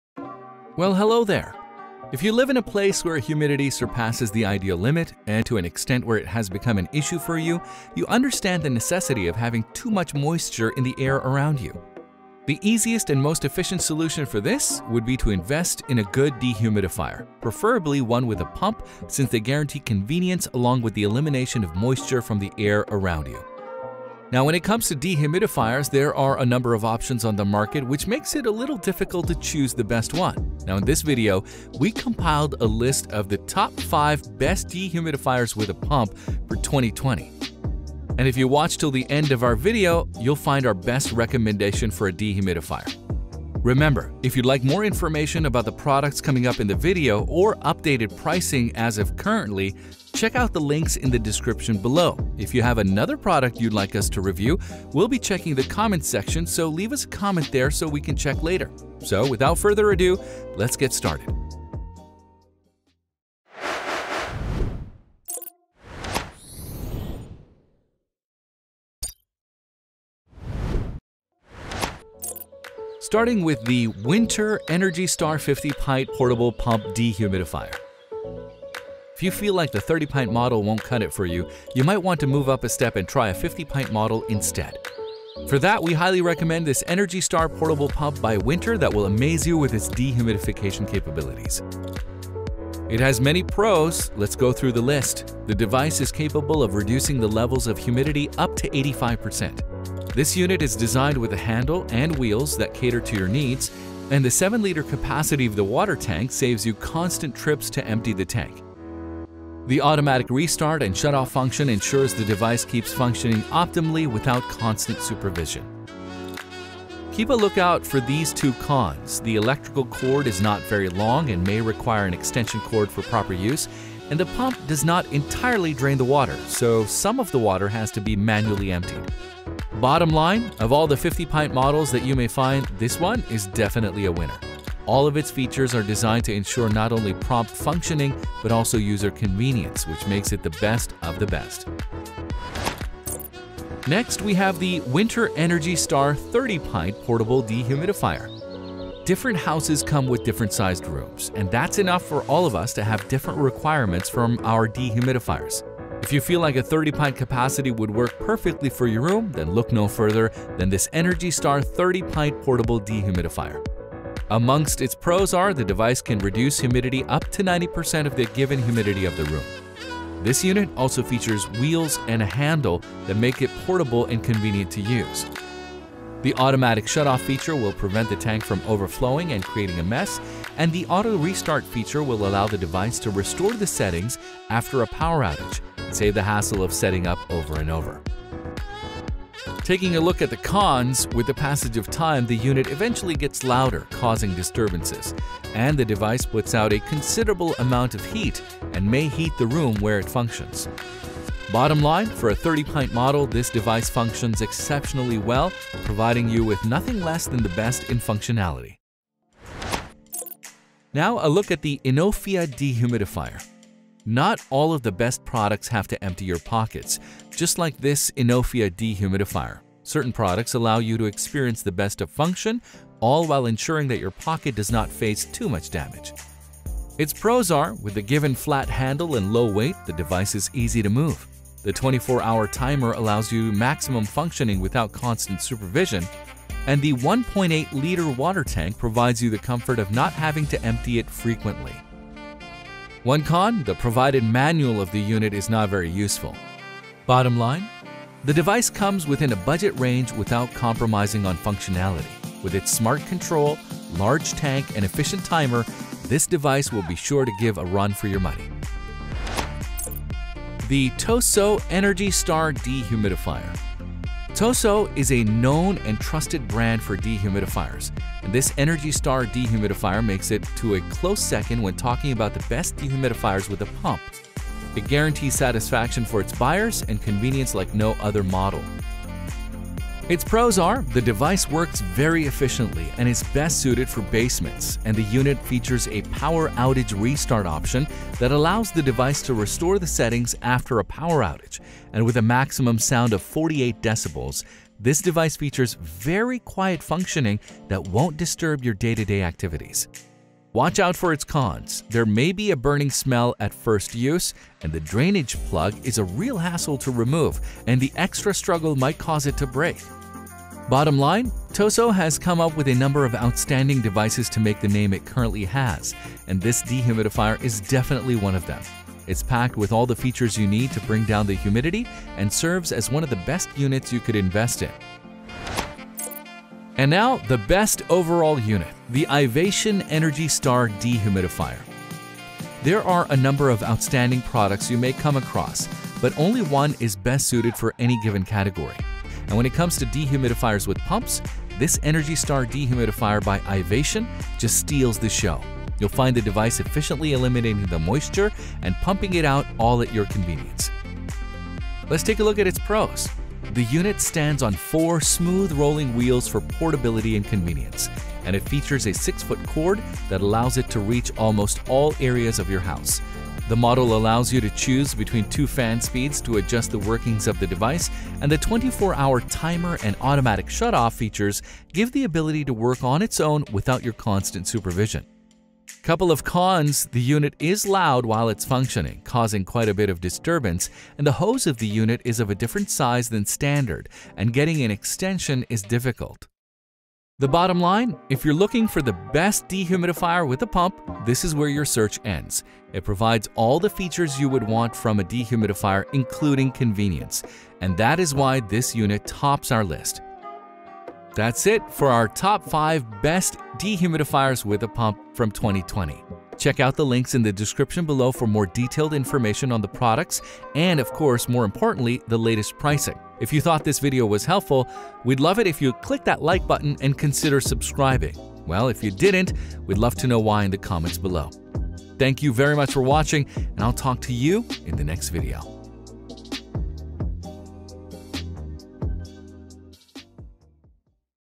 Best Dehumidifiers With Pump 🌫: Top Options Reviewed | HVAC Training 101
(Best Dehumidifiers With Pump)

✅ Featured Products for Best Dehumidifiers With Pump

0:00 Introduction
1:13 5.Whynter Energy Star 50 Pint ➜
2:25 4.Whynter Energy Star 30 Pint ➜
3:32 3.Inofia Dehumidifier ➜
4:21 2.TOSOT Energy Star ➜
5:27 1.Ivation Energy Star ➜

🏡 If you live in a place where humidity surpasses the ideal limit, and to an extent where it has become an issue for you, you understand the necessity of having too much moisture in the air around you.

👌 The easiest and the most efficient solution for this would be to invest in a good dehumidifier, preferably one with a pump since they guarantee convenience along with the elimination of moisture from the air around you.

💬 Let us know what you think of these Dehumidifiers With Pump in the comment section below.